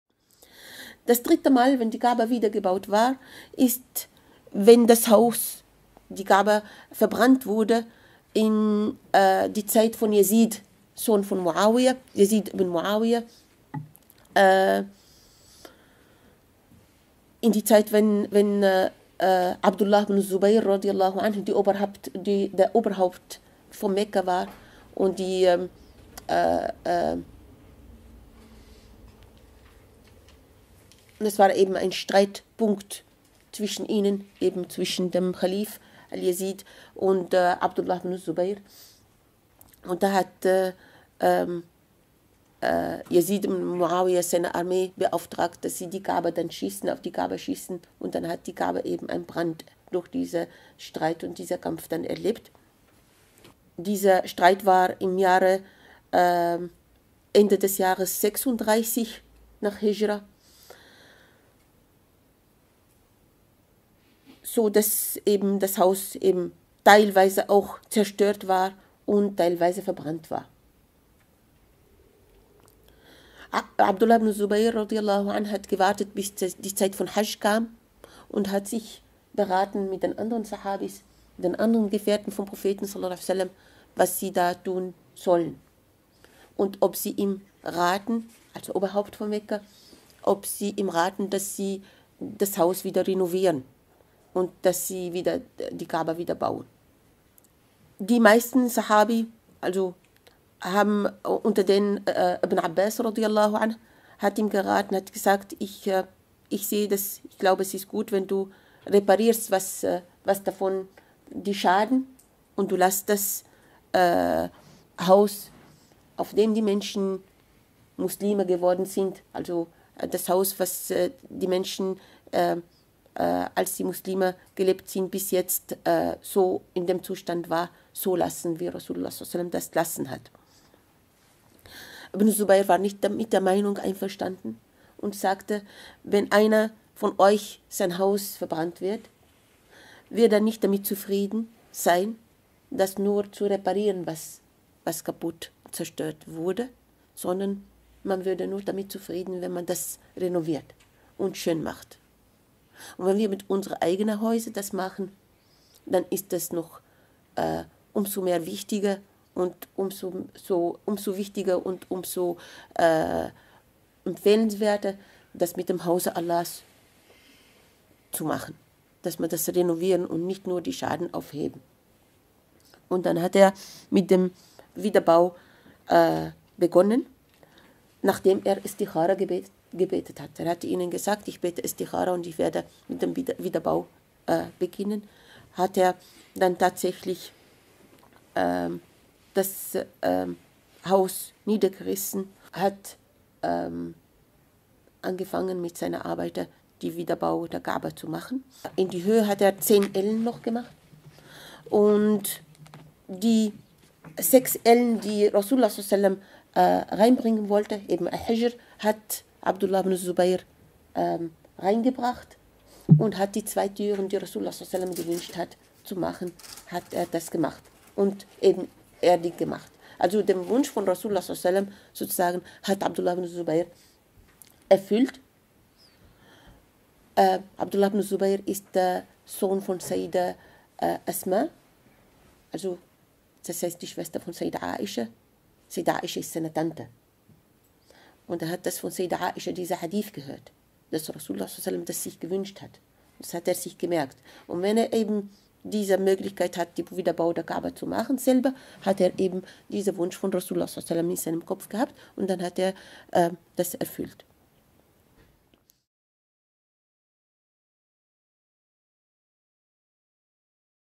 Der 3. Wiederaufbau der Ka'ba durch Abdullah ibn al-Zubayr - Ustadha Mariem Dhouib
Madrasah - Verein für islamische Bildung und interkulturellen Dialog